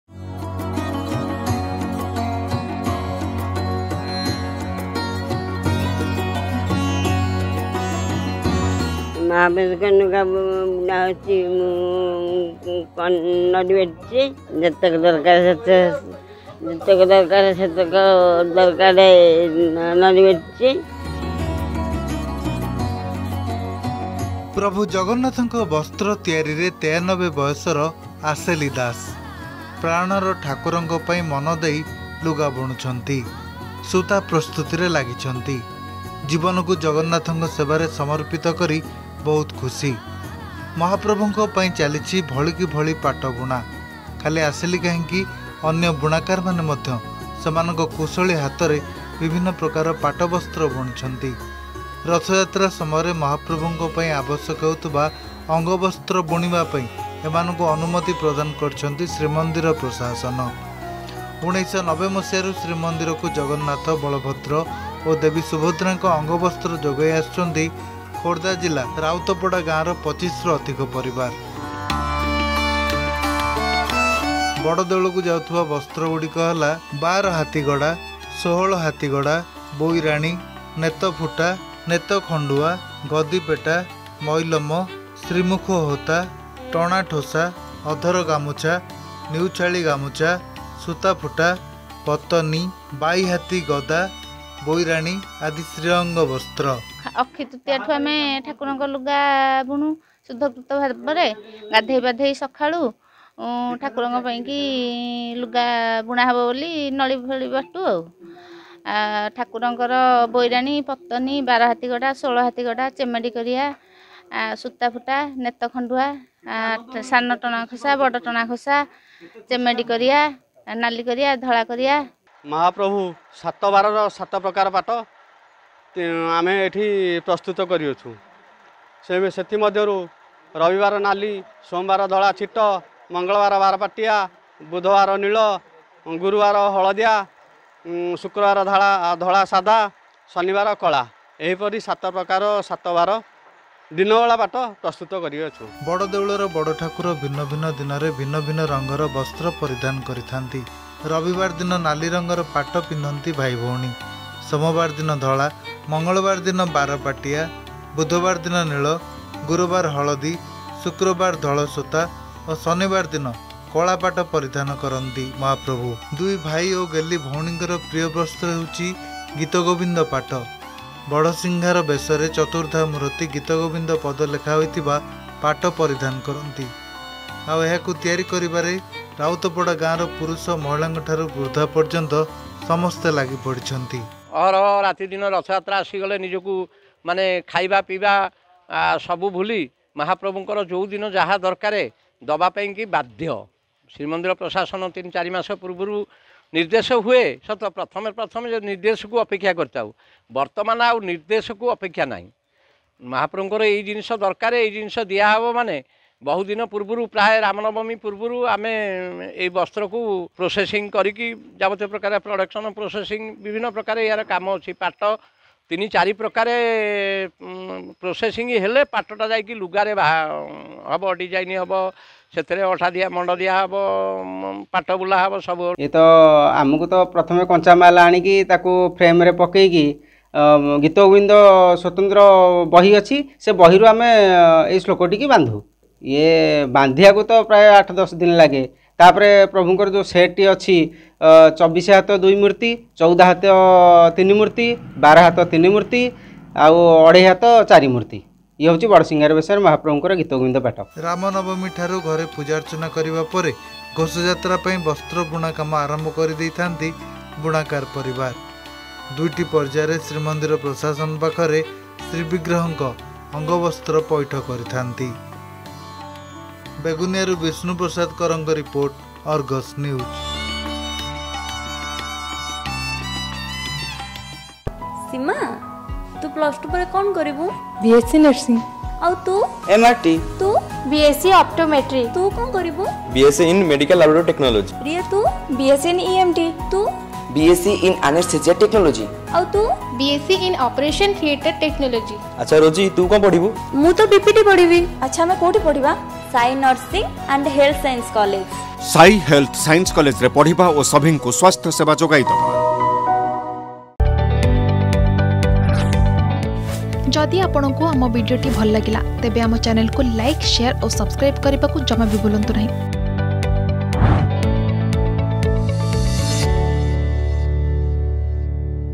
ମହାପ୍ରଭୁଙ୍କ ପାଇଁ ଚାଲିଛି ଭଳି କି ଭଳି ପାଟ୍ଟ ବୁଣା | Weavers Preparing Sri Anga Vastra For Lord Jagannath
ପାଖେଇ ଆସୁଛି ଘୋଷଯାତ୍ରା । ମହାପ୍ରଭୁଙ୍କ ବସ୍ତ୍ର ତିଆରିରେ ୯୩ ବୟସରେ ଆସେଲି ଦାସ । ଜୀବନକୁ ଜଗନ୍ନାଥଙ୍କ ସେବାରେ ସମର୍ପିତ କରି ବହୁତ ଖୁସି । ମହାପ୍ରଭୁଙ୍କ ପାଇଁ ଚାଲିଛି ଭଳି କି ଭଳି ପାଟ୍ଟ ବୁଣା । ଜଗା କାମରେ ନିଜକୁ ନିୟୋଜିତ କରିଛନ୍ତି ଖୋର୍ଦ୍ଧା ଜିଲ୍ଲା ବେଗୁନିଆ ବ୍ଲକ ରାଉତପଡା ଗ୍ରାମର ବୁଣାକାର ପରିବାର ।